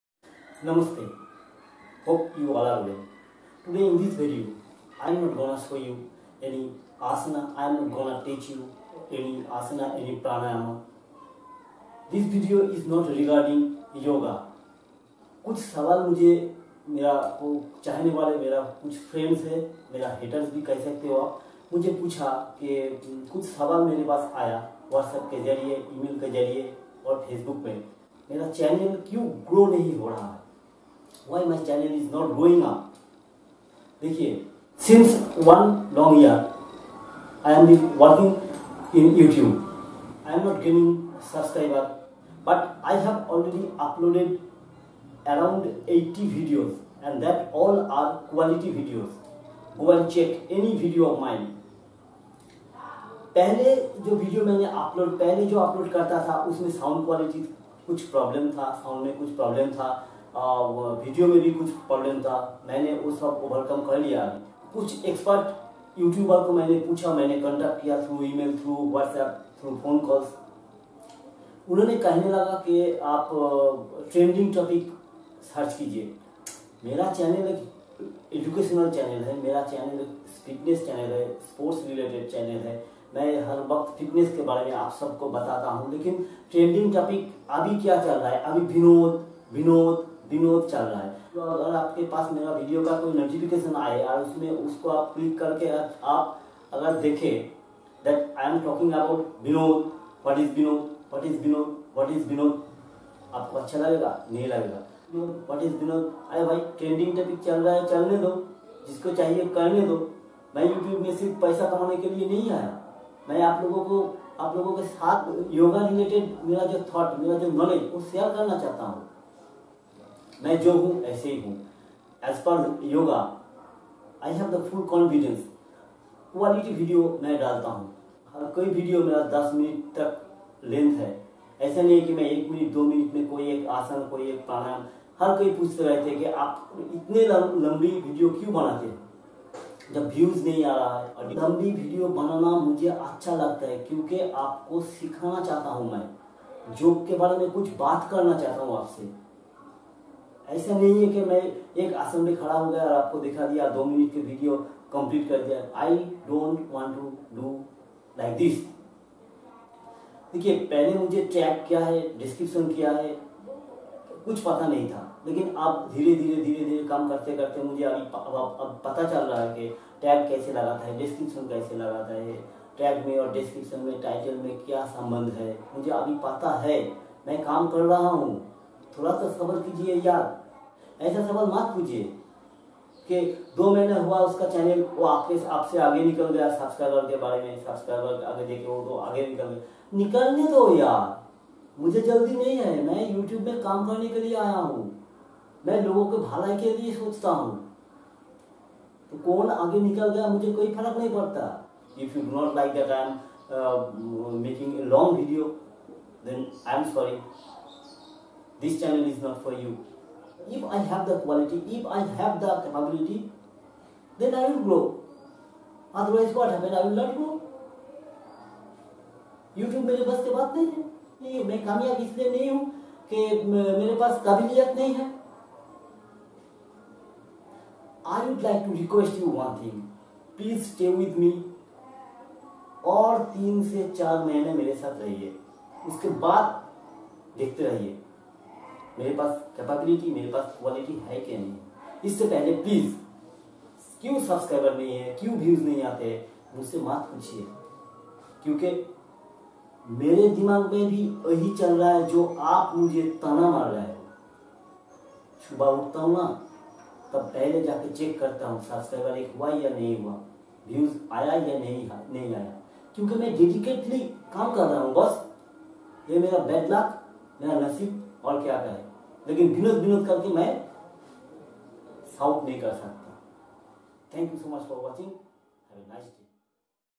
I am gonna quit YouTube || Binod , yes BINOD ke liye
Sorry !! yaaro,  I am really really upset about your queries, why im not getting views in my videos! Why after one year my subscribers count are not up to the mark?
I dont know really, Some YouTube expert says go for trending topics as of now BINOD is trending , they suggest me to make video on binod, how it is possible for me ??
I am a Yogi and I teach how to get fit by doing yoga , how BINOD comes in this? I even dont know who is binod? Why Binod is trending ?